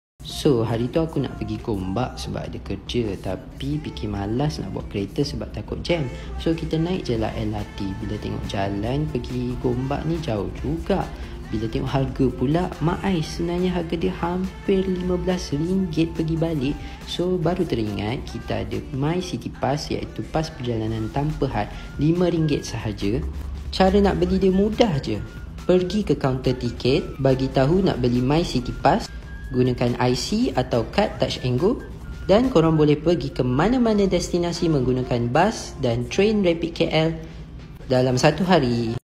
Tips Jimat Naik LRT MRT Monorel & BRT RM5 Sehari! Ini Cara Beli Mycity Pass RM5 Perjalanan Tanpa Had
Tips Jimat Naik LRT MRT Monorel Dan BRT RM 5 Je Sehari!